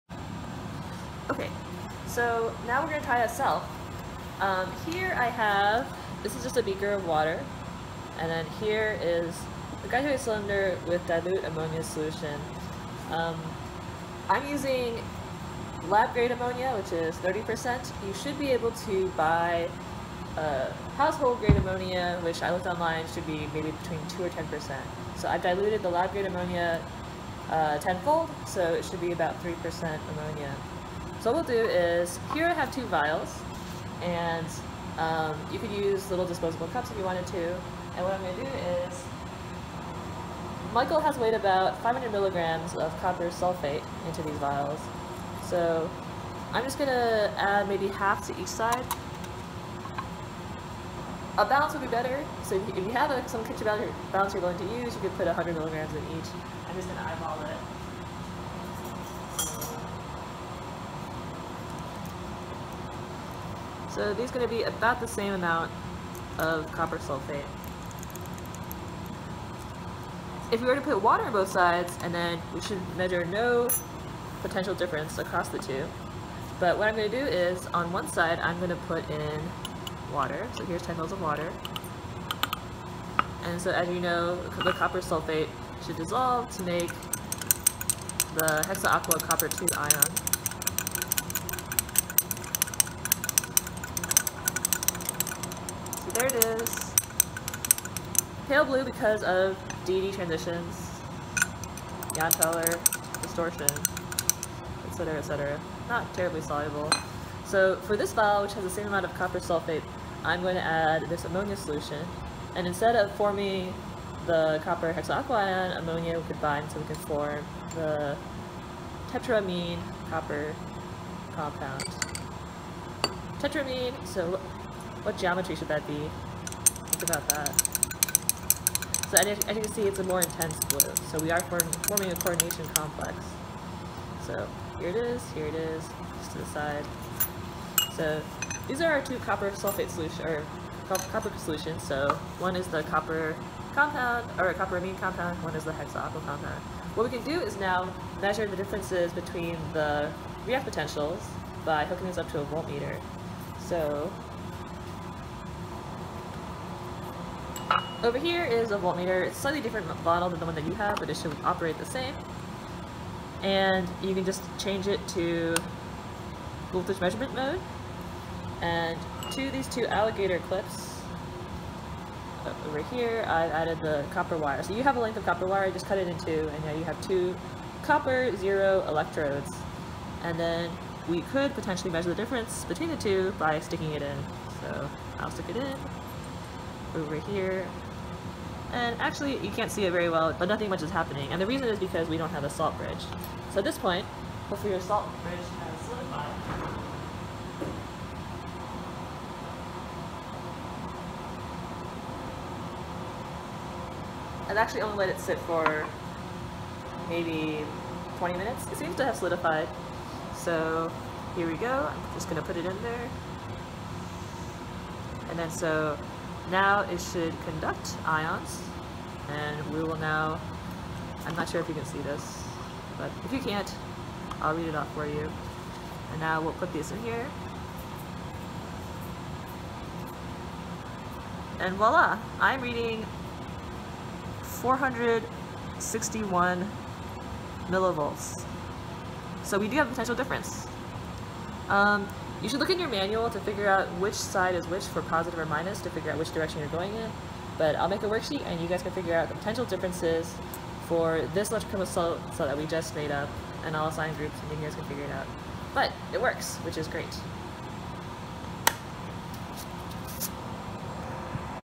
Electrochemistry Kit: Copper Experiment
Making a copper/copper ion electrochemical cell.

CHEM 20284
Spring 2020